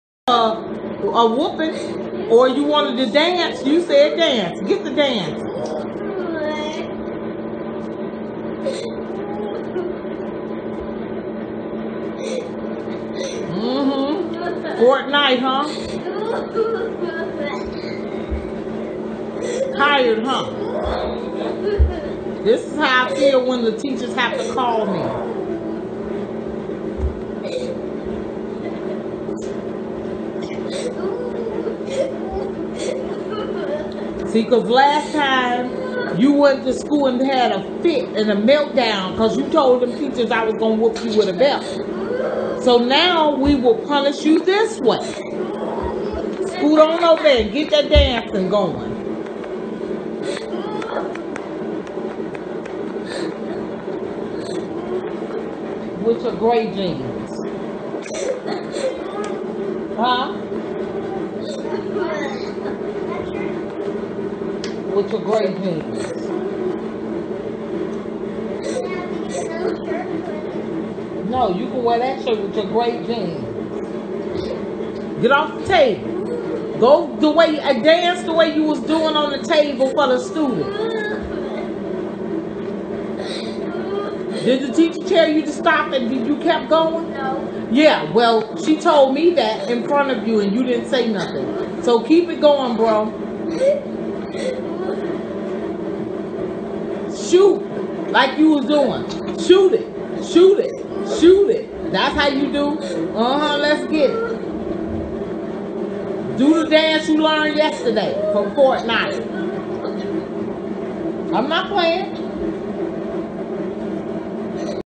Kid Dance For Punishment
Kid gets in trouble at school for dancing but feared to get a spanking from mom. Instead the kid chose to dance over a whipping but gets the workout he wasn't expecting.